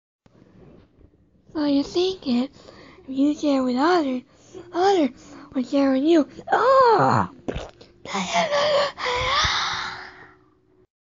Big Bird's Egg | Bird TV | Ep.4
Big Bird Has Accidentally Lays An Egg When Talking To Kids About Sharing.